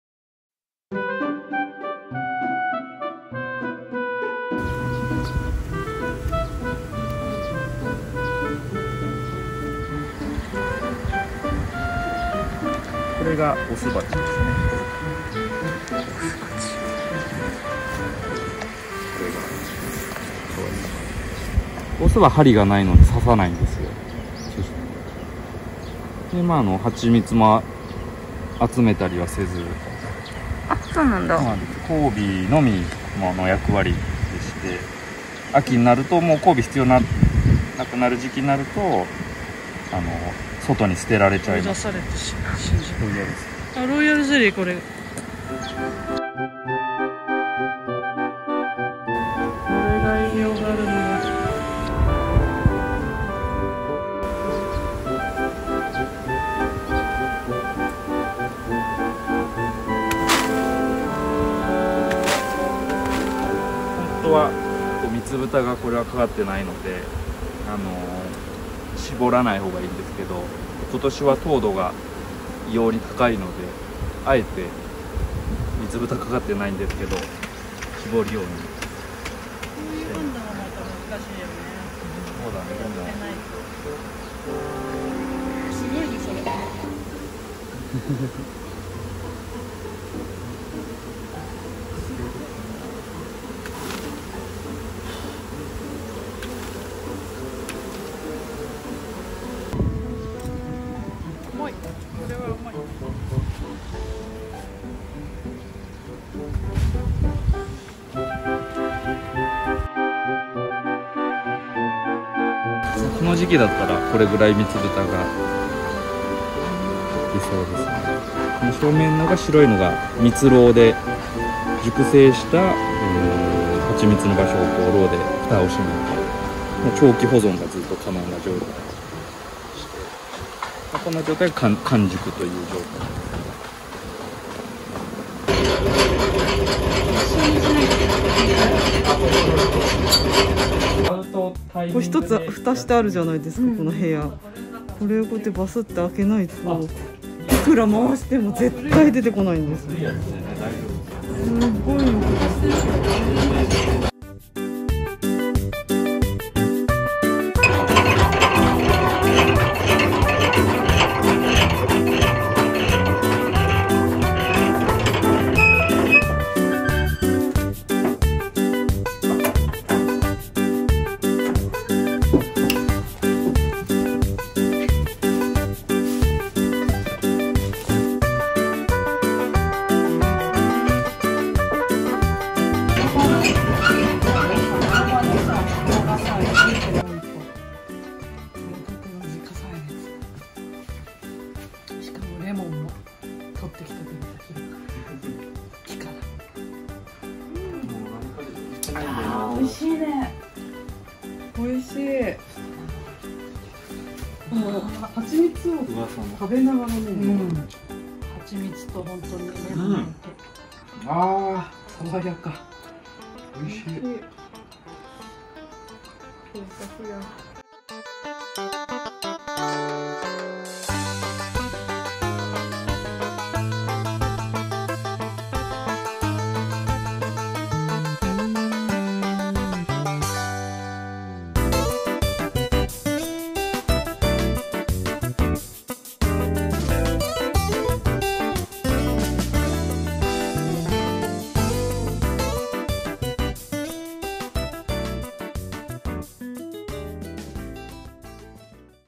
湘南のはちみつ屋さん honobono工房 採蜜2020
湘南（茅ヶ崎）のはちみつ屋さん honobono工房さんの今年初めての採蜜の様子。